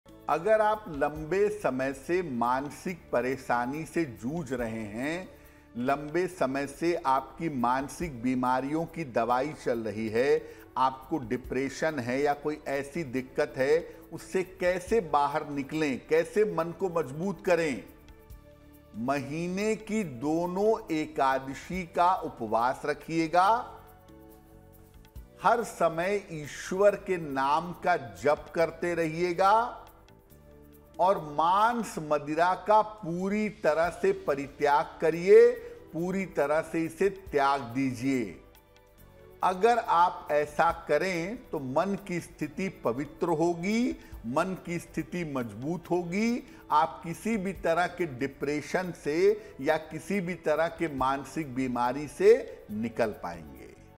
Aaj Ka Upay 20 मई 2023: लंबे समय से मानसिक परेशानी से जूझ रहे हैं, करें ये उपाय | Aaj Ka Jyotis
Aaj Ka Jyotish Upay 20 मई 2023: इस वीडियो में ज्योतिष शैलेंन्द्र पांडेय बता रहे हैं कि अगर आप लम्बे समय से मानसिक परेशानी से जूझ रहे है, तो महीने की दोनों एकादशी का उपवास रखें. हर समय ईश्वर के नाम का जप करते रहें. मांस मदिरा का सम्पूर्ण परित्याग करें.